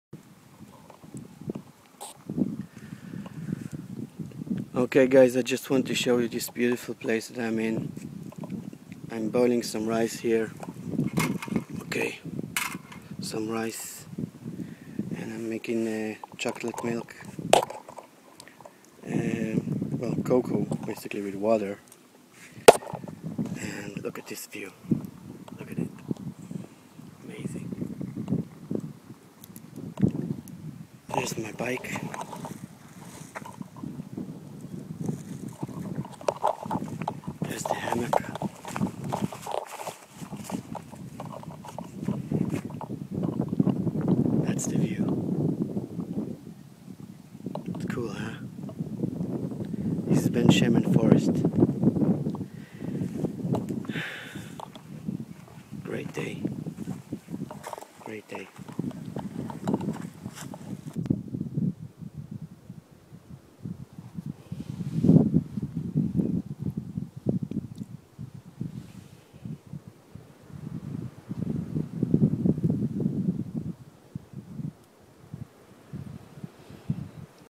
A graet day at Ben Shemen forest
Ben Shemen forest is one of the largest forests in Israel, and it is 20 min drive from my house. I packed a net hammock and my cocking kit along and solo ride the single tracks of the forest then camped below an olive tree made some rice and hot chocolate and shared  this view and great time with you.Hope you enjoy.